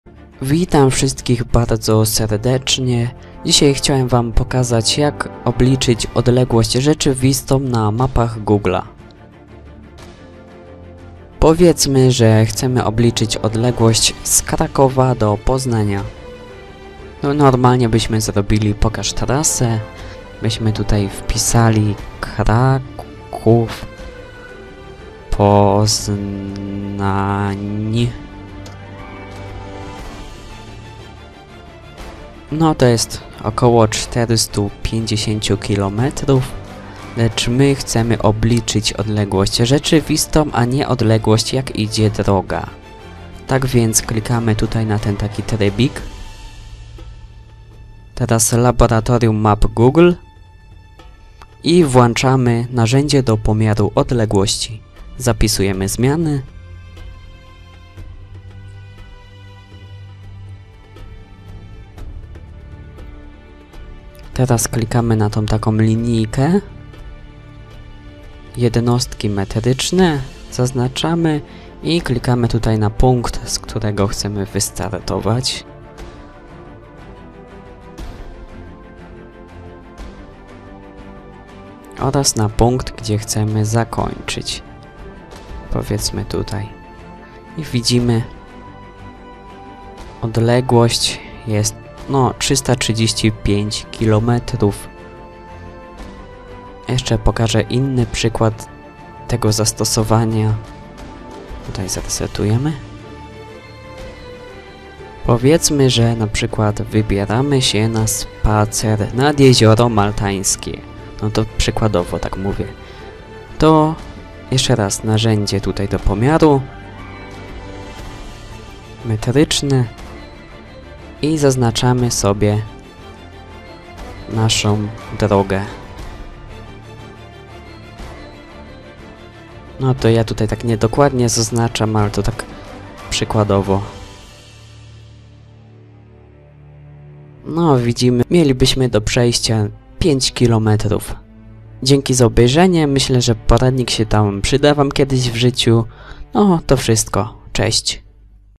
Jak obliczyć odległość rzeczywistą na mapach Google
Odległość rzeczywista czyli przez łaki, pola, lasy :P